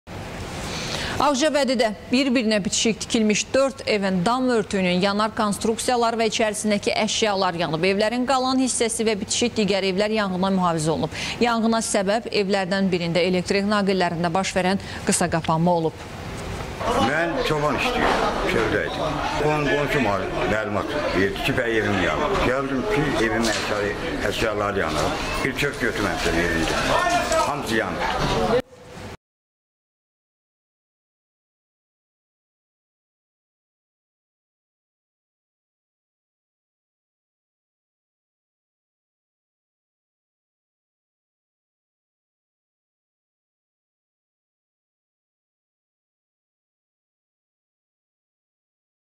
Ağcabədidə bir birinə bitişik dörd ev yandı
Ağcabədidə bir-birinə bitişik tikilmiş 4 evin dam örtüyünün yanar konstruksiyaları və  içərisindəki əşyalar yanıb. Evlərin qalan hissəsi və bitişik digər evlər yanğından mühafizə olunub. Yanğına səbəb evlərdən birində elektrik naqillərində baş verən qısaqapanma olub